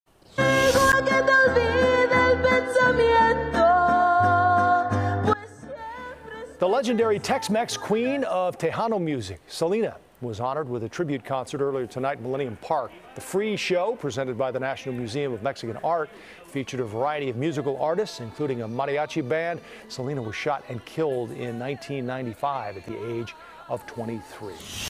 Selena tribute held at Millennium Park
The free show was presented by the National Museum of Mexican Art.